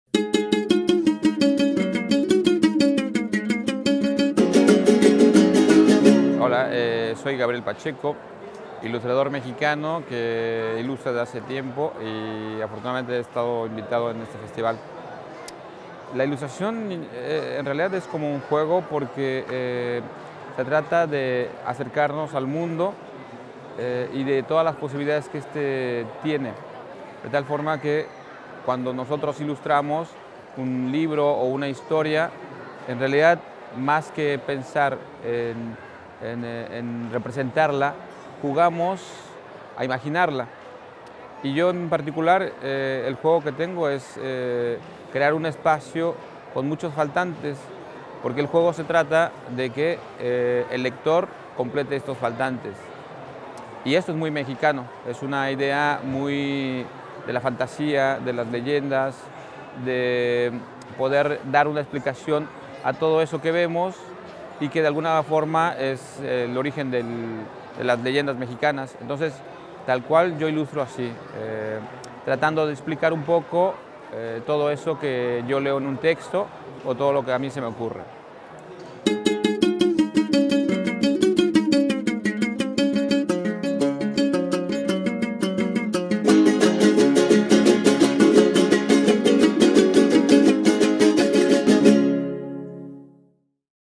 Gabriel Pacheco
Voci del Festival
Intervista a Gabriel Pacheco

Tocatì | 18.19.20 e 21 settembre 2014
Verona, Centro Storico